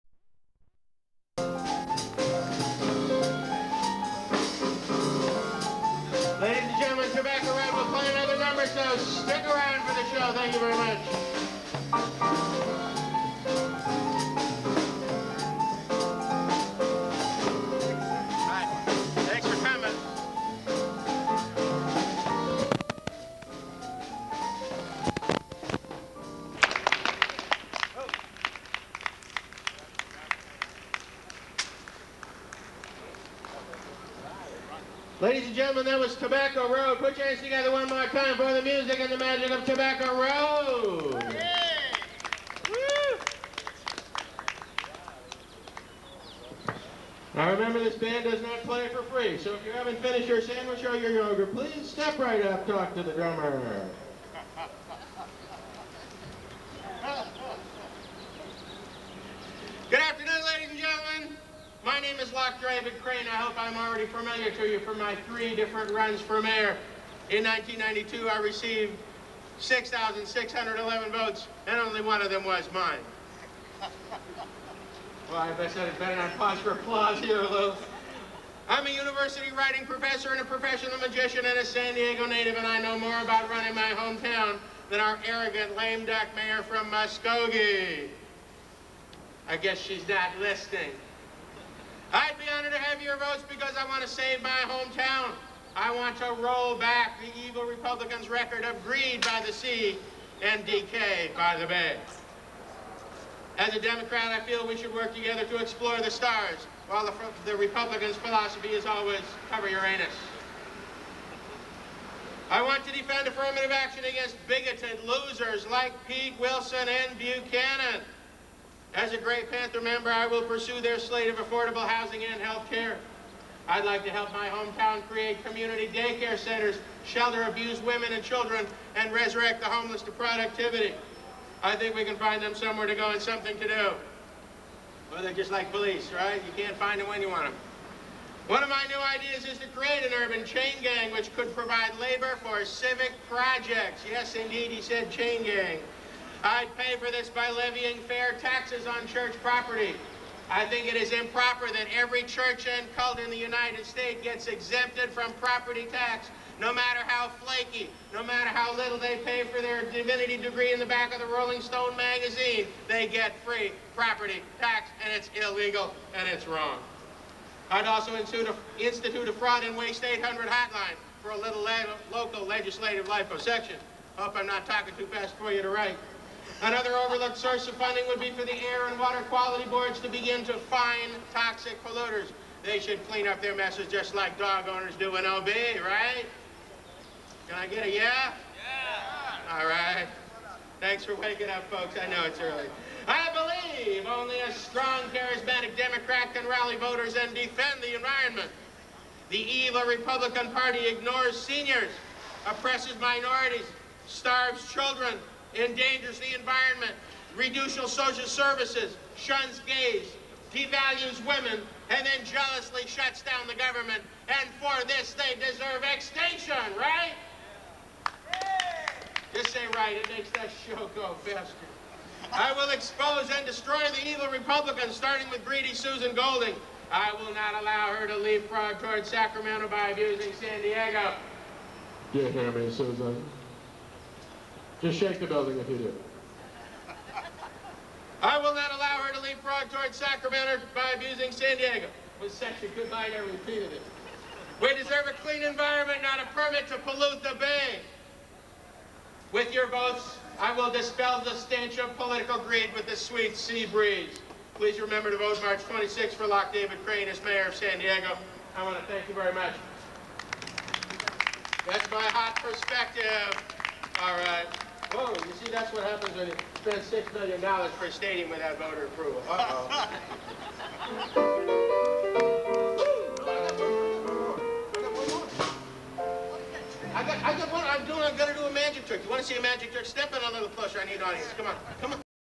Crane, Democrats rail against Queen Golding in 1996
Loch David Crane's extra long mayoral speech was delivered at City Hall outside rally.  All the big hits and more:   tax church property, support Gray Panthers, the evil Republican party deserves extinction, misdemeanor  holding facility, urban chain gang for civic projects...and a live band.